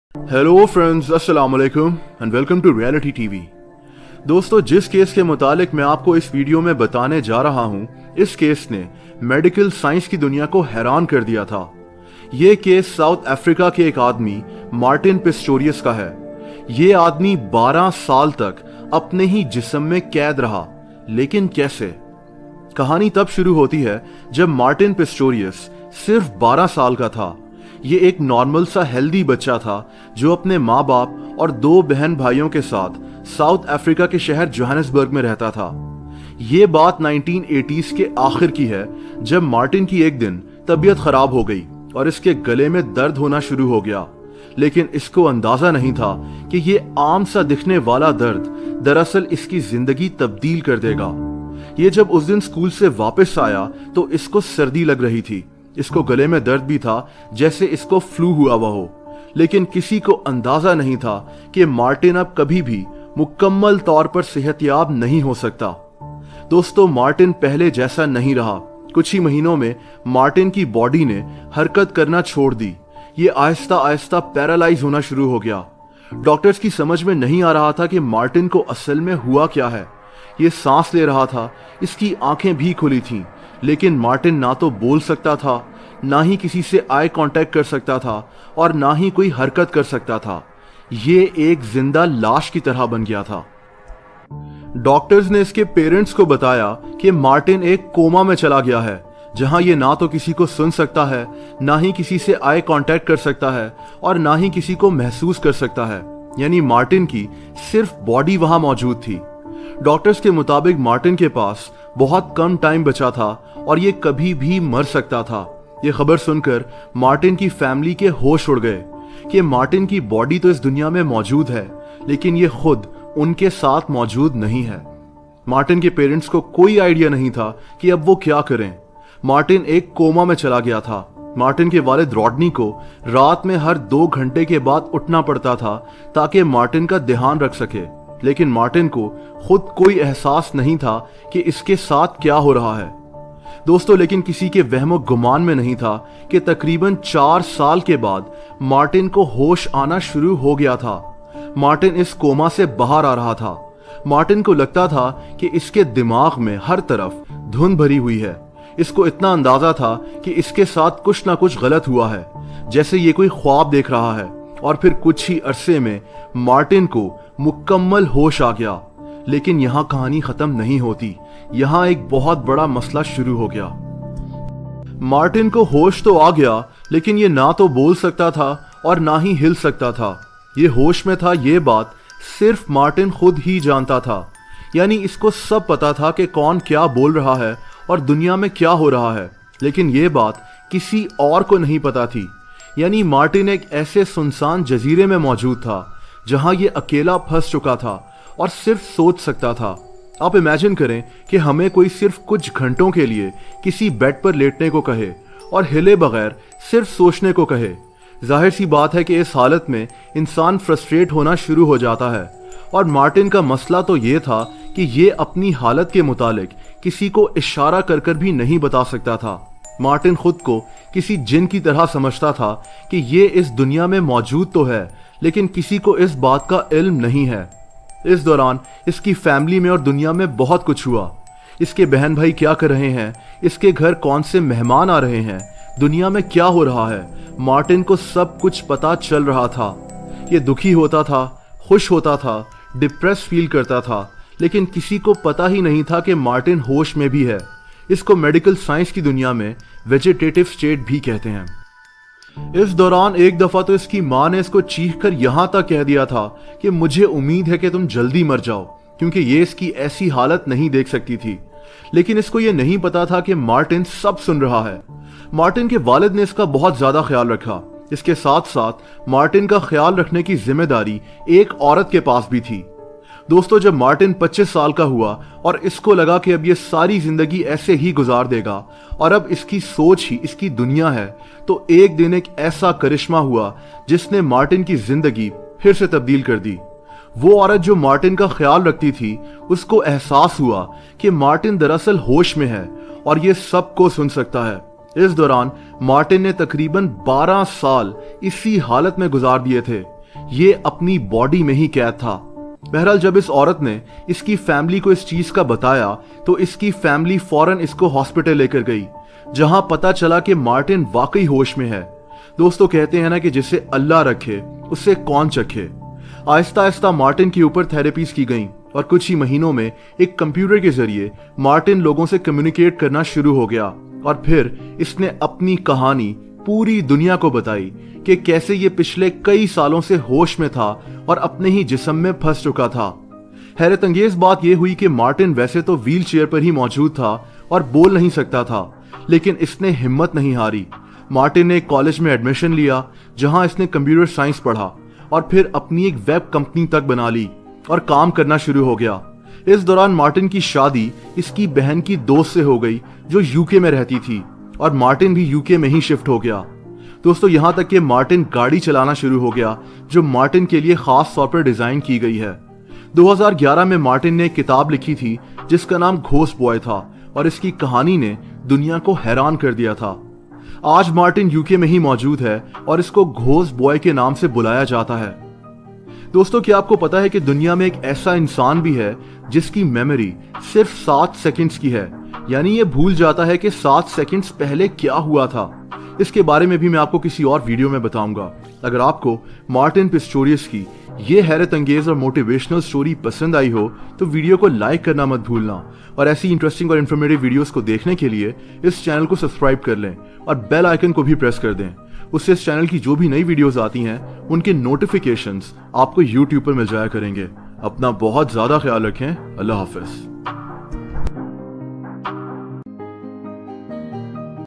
Story of Martin Pistorius Boy Who Was Trapped in his Own Body for 12 Years YouTube
This Video is about the story of Martin Pistorius who fell into a mysterious coma but after 12 years he revealed his strange story to the world